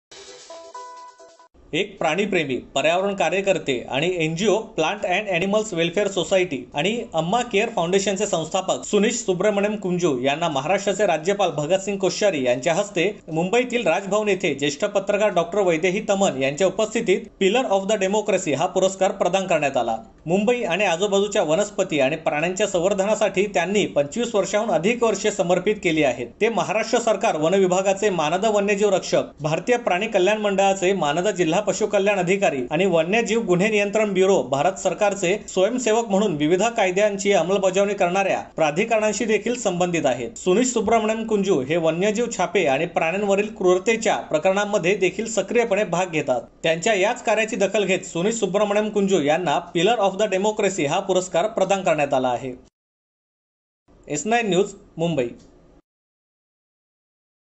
S9 TV News | 2021 Dec. 08 | भांडुपचे निसर्ग - प्राणि मित्र सुनिष कुंजु यांचा राज्यपालांनी केला गौरव
S9 TV News Bulletin | 2021 December 08 Wednesday | भांडुपचे निसर्ग - प्राणि मित्र सुनिष कुंजु यांचा राज्यपालांनी केला गौरव

एक प्राणी प्रेमी, पर्यावरण कार्यकर्ते आणि एनजीओ प्लांट अँन्ड अनिमल्स वेलफेयर सोसायटी- मुंबई (पीएडब्ल्यूएस-मुंबई) आणि अम्मा केअर फाऊंडेशन (एसीएफ) चे संस्थापक सुनिष सुब्रमण्यन कुंजु यांना महाराष्ट्राचे माननीय राज्यपाल भगतसिंग कोश्यारी यांच्या हस्ते मुंबईतील राजभवन येथे ज्येष्ठ पत्रकार डॉ. वैदेही तमन यांच्या उपस्थितीत "पिलर ऑफ द डेमोक्रेसी" पुरस्कार प्रदान करण्यात आला. मुंबई आणि आजूबाजूच्या वनस्पती आणि प्राण्यांच्या संवर्धनासाठी त्यांनी 25 वर्षांहून अधिक वर्षे समर्पित केली आहेत. ते महाराष्ट्र सरकार, वन विभागाचे मानद वन्यजीव रक्षक, भारत सरकार, भारतीय प्राणी कल्याण मंडळाचे मानद जिल्हा पशुकल्याण अधिकारी आणि वन्यजीव गुन्हे नियंत्रण ब्युरो, भारत सरकारचे स्वयंसेवक म्हणून विविध कायद्यांची अंमलबजावणी करणार्या प्राधिकरणांशी देखील संबंधित आहेत. ते वन्यजीव छापे आणि प्राण्यांवरील क्रूरतेच्या प्रकरणांमध्ये सक्रियपणे भाग घेतो.